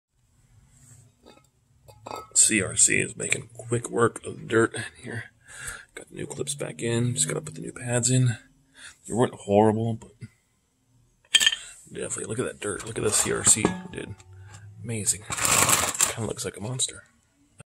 New wagner brake pads ✊😎
Wagner oex pads ...they are amazing and smooth and amazing stopping power was recomemded these now im doing the same..as you can see still had some pads but werent feeling right ...shout out to crc brake cleaner for making quick work of the dirt